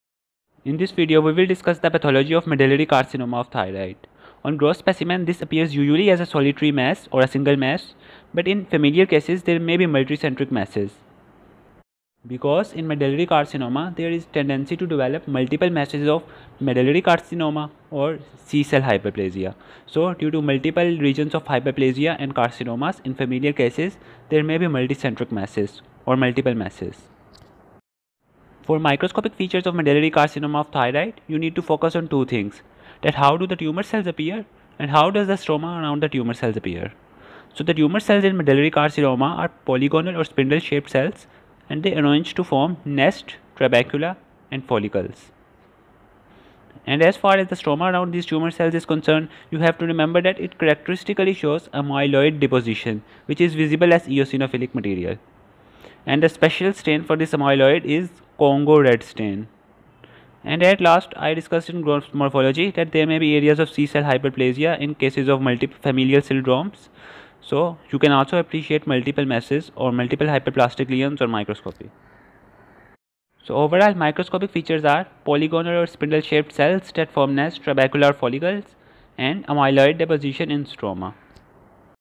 Medullary Carcinoma Of Thyroid (Morphology)
In this video, we will discuss the pathology of medullary carcinoma of thyroid.
Medullary carcinoma of thyroid can arise in a familial or sporadic way. It is characterized by presence of C cells and Amyloid stroma.